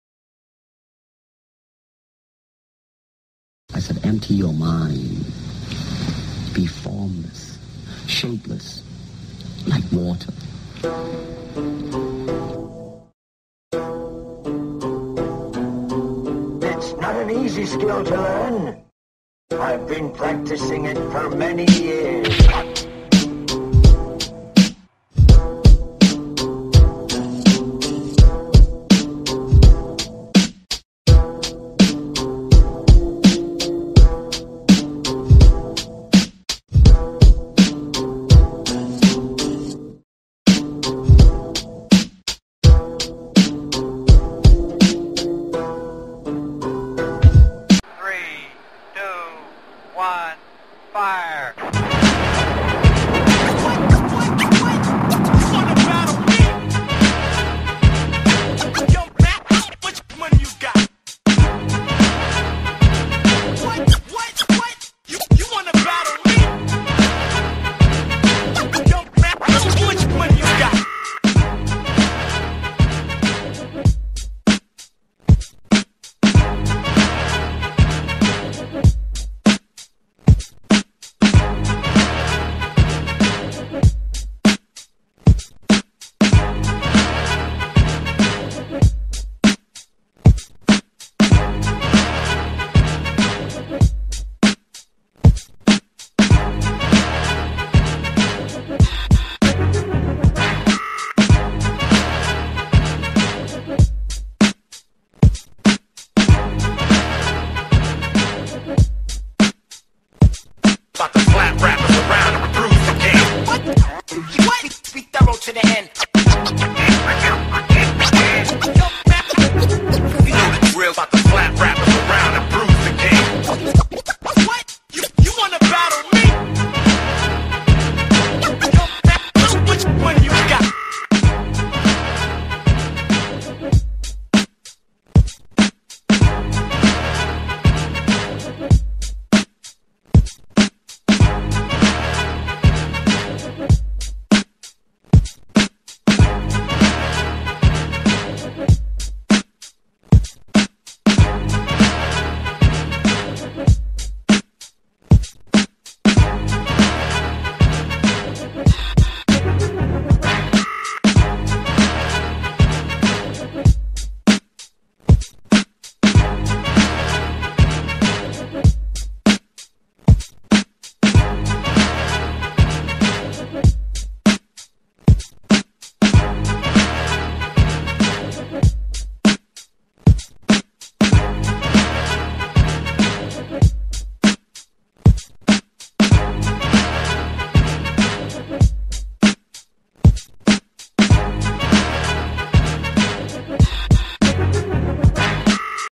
Arizona Swimbait Style
Arizona Swimbait Fishing for the big bass of the desert southwest, saguaro lake and canyon lake